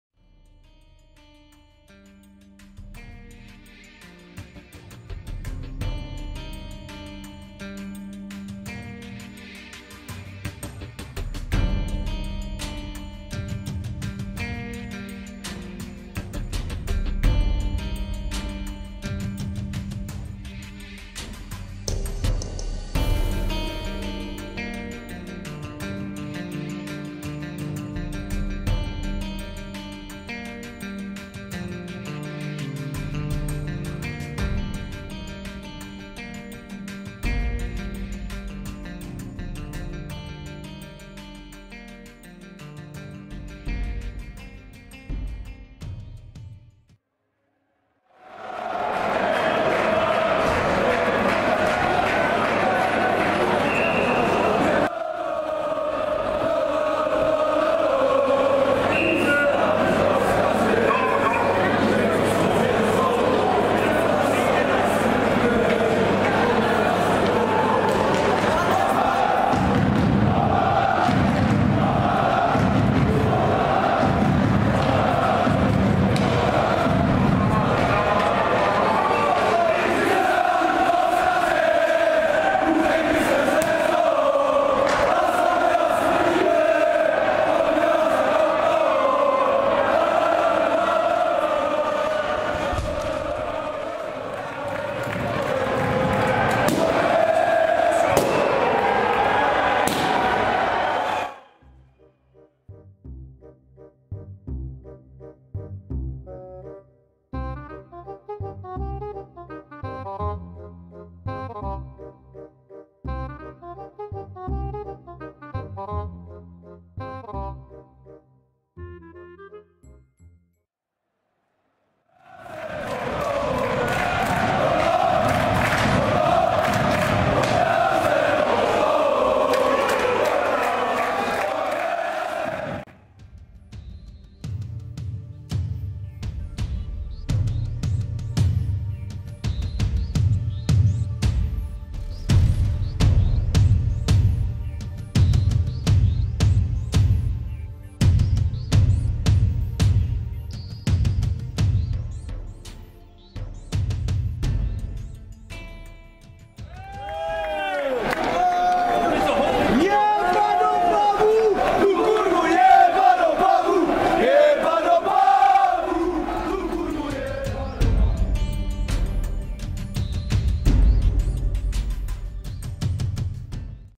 Slezan Opava-AZ Havířov (2. ČF) -Prezentace na obou stranách + pyro na konci ... 4. 3. 2013
V Opavě se prezentovali oba sektory a domácí si připravili pyrotechniku na konci.

Série se pak po výsledku 3:0 pro domácí srovnala na 1:1.